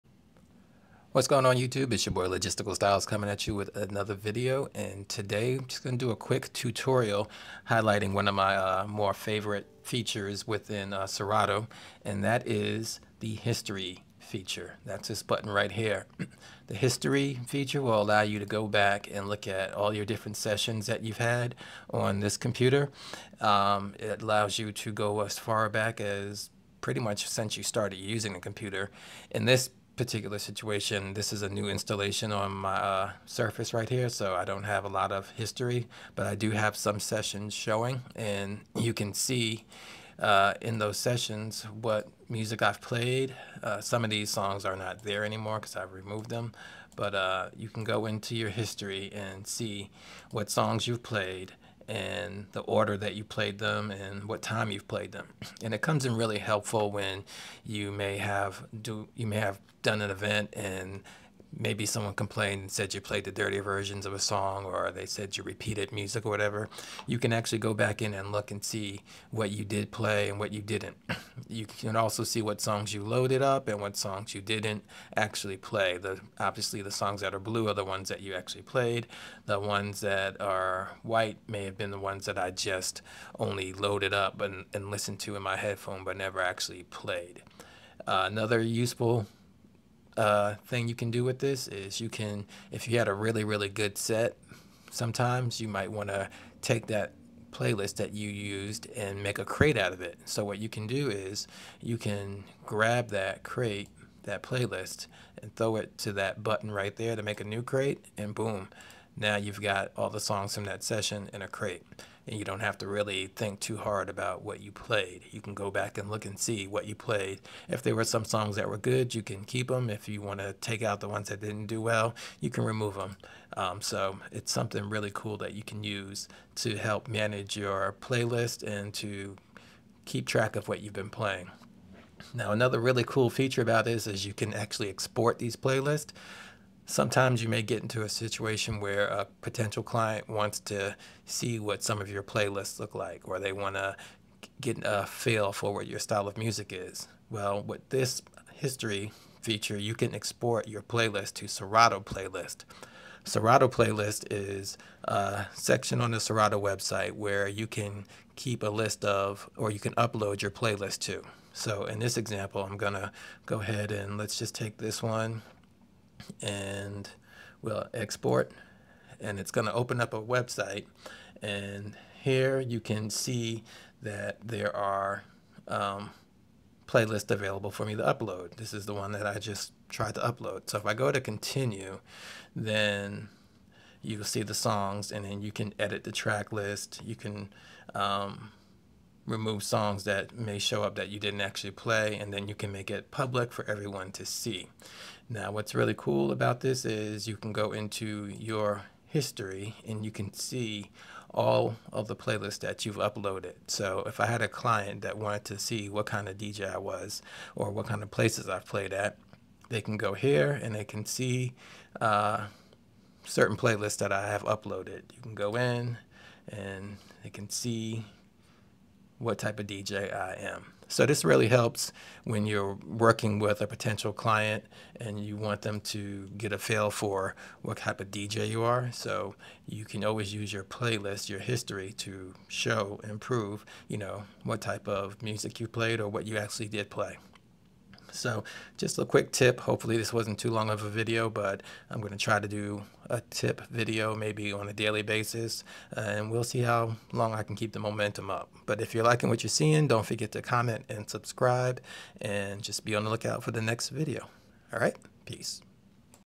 Using the History Feature in Serato DJ Pro
The History feature in Serato is very useful and probably not used as much as it should be. This is a quick tutorial on how I use it and why you should as well.